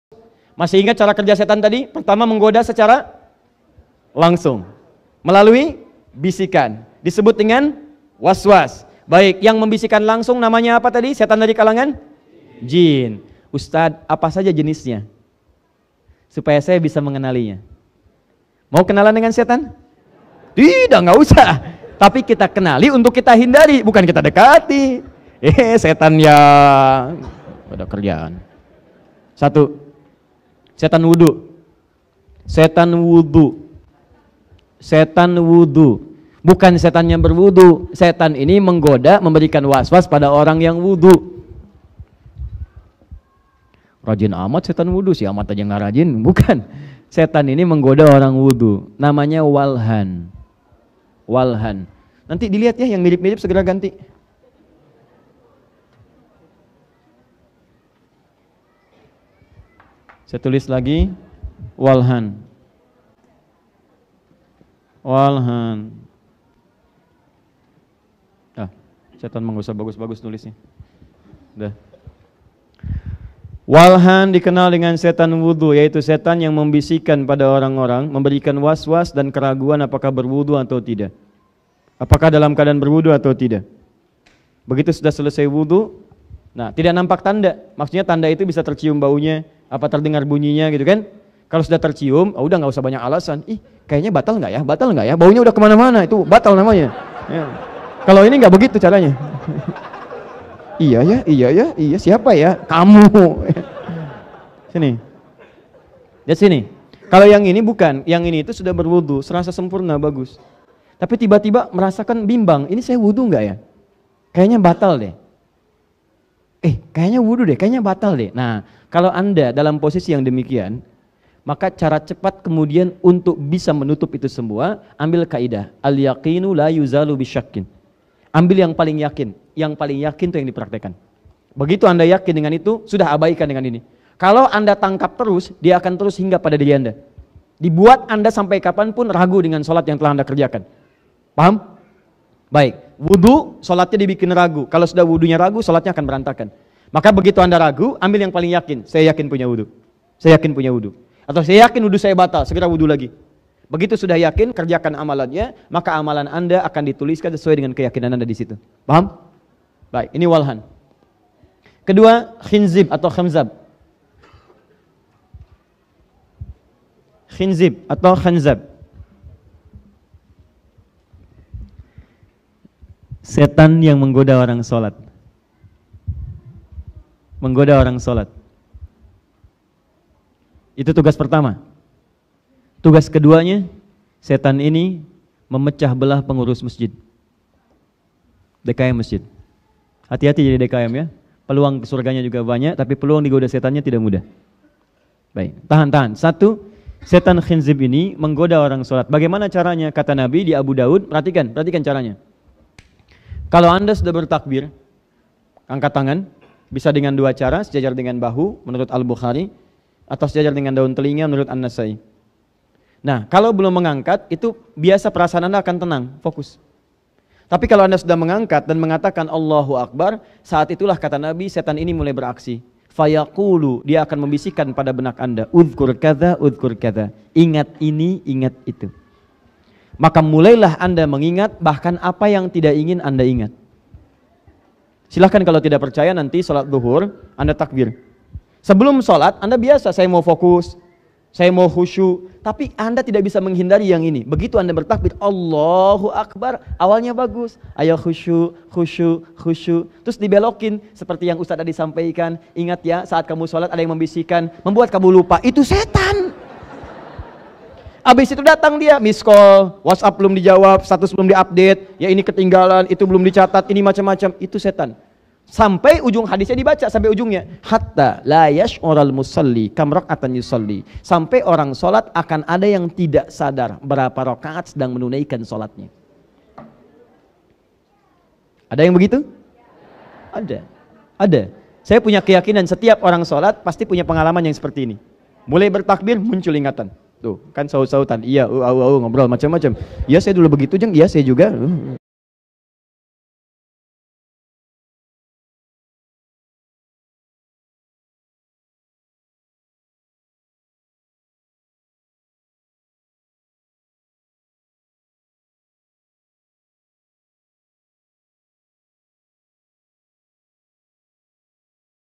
Ceramah Ustadz Adi Hidayat Godaan Setan saat sholat dan wudhu
Assalamualaikum warahmatullahi wabarakatuu
Semoga apa yang kami bagikan semua dapat bermanfaat dan bernilai Ibadah bagi kita semua